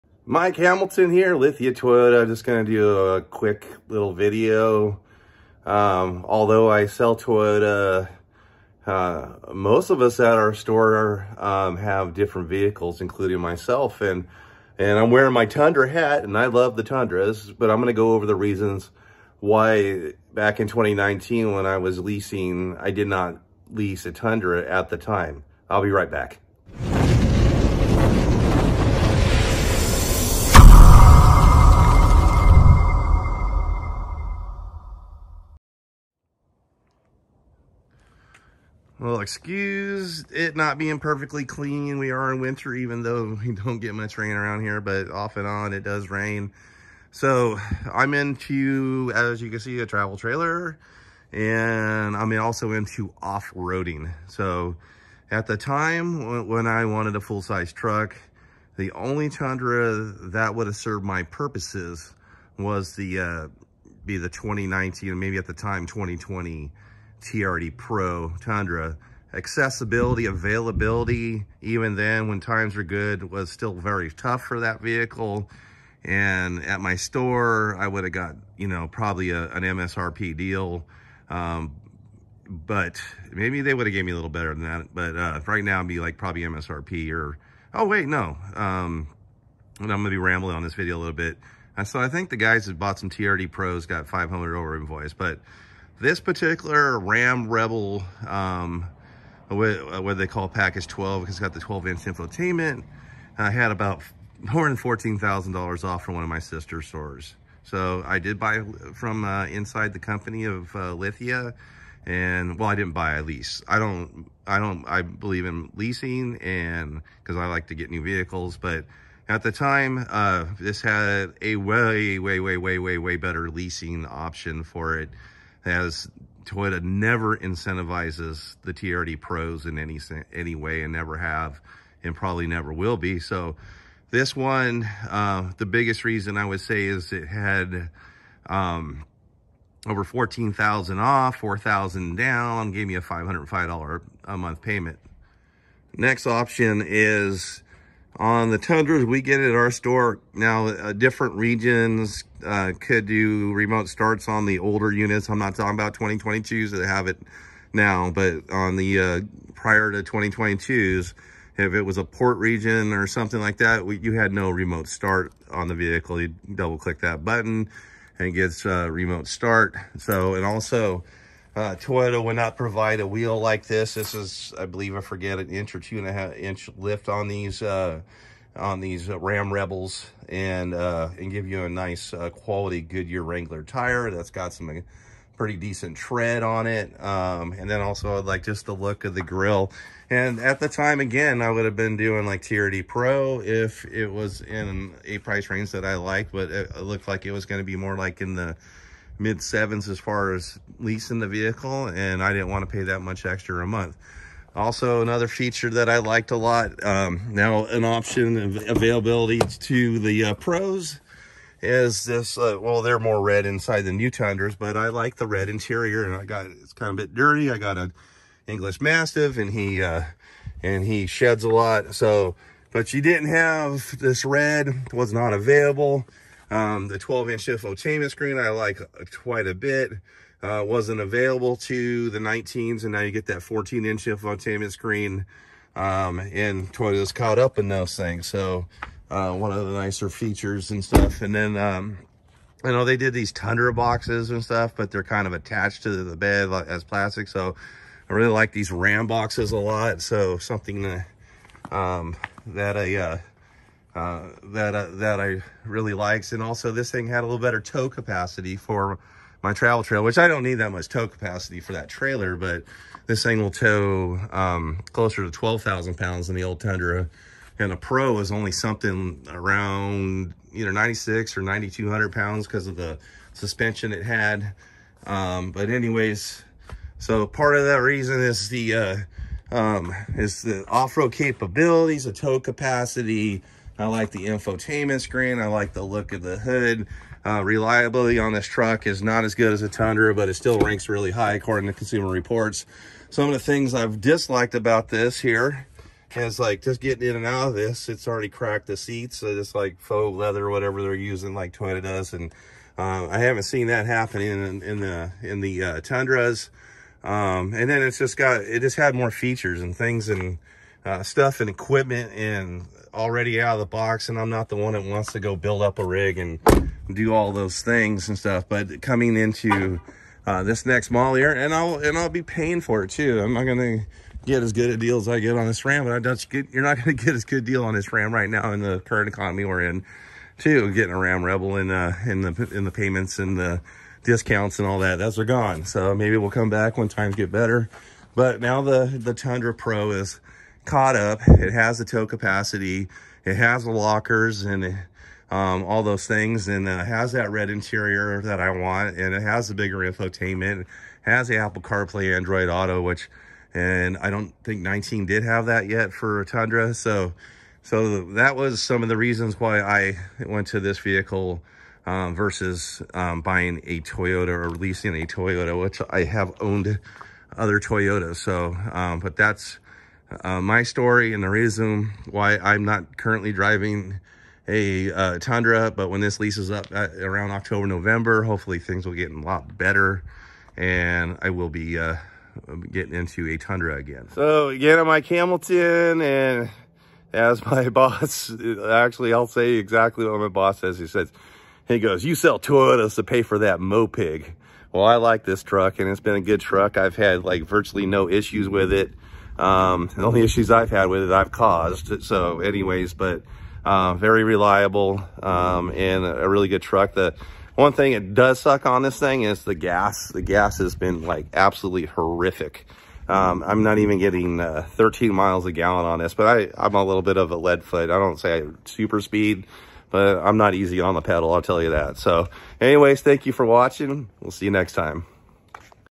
Why the Toyota Salesman doesn’t drive a Toyota…
Why I picked a RAM Rebel in 2019 and not a Toyota Tundra. Here is my story why…